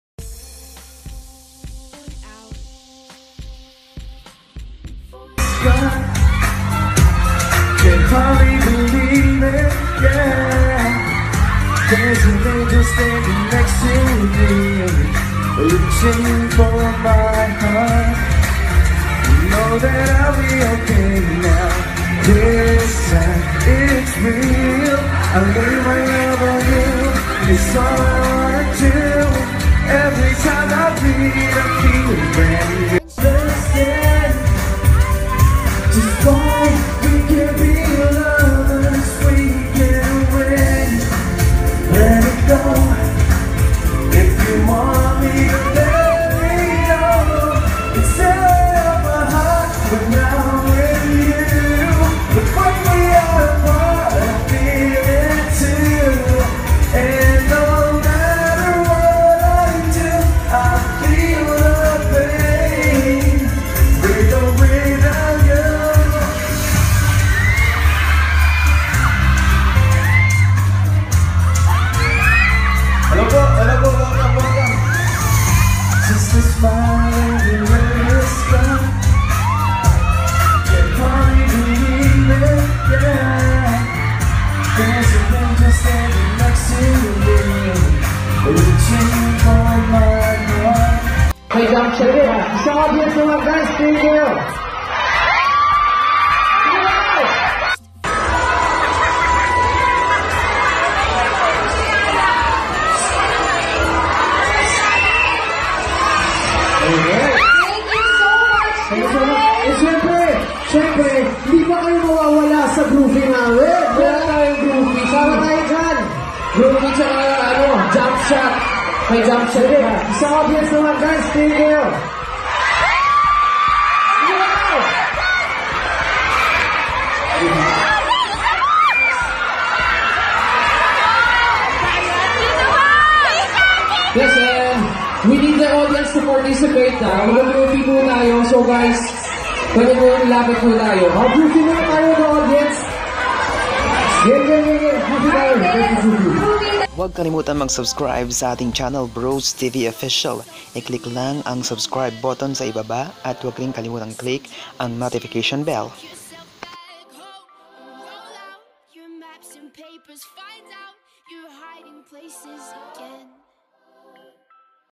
"Bigyan ng Jacket Yan Portion" ni Alden Richards in Kapuso Grandsfans Day!
Panoorin ang nakaka kilig na performance ni Alden Richards at ang kanyang bigyang ng Jacket Portion!